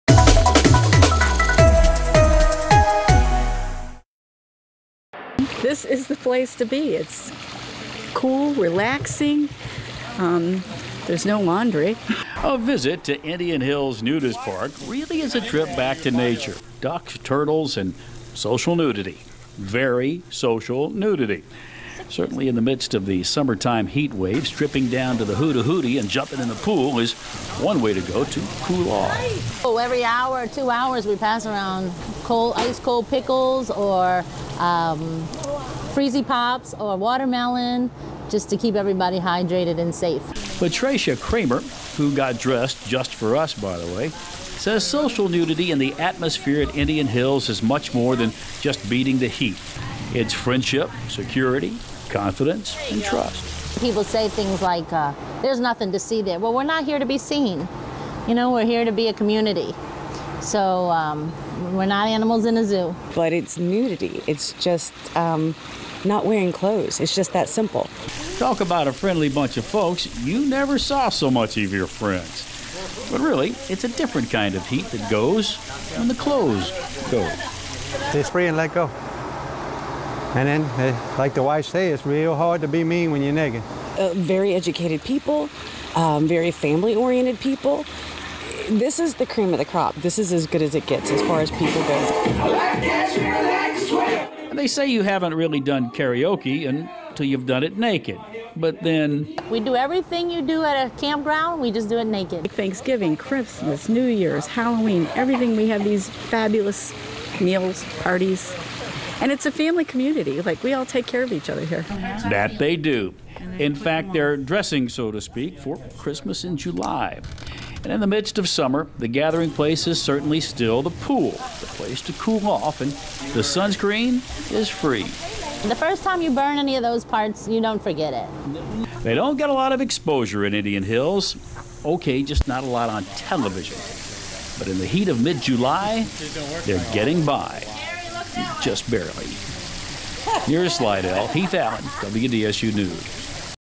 Naked Truth: Nudists don't sweat on ways to keep cool during summer heat
Owners of the Indian Hills Nudist Park near Slidell, Louisiana, say the park is much more than just a place to get naked and cool off in the pool. It's a place to enjoy friendships and gain confidence in a safe, secure environment.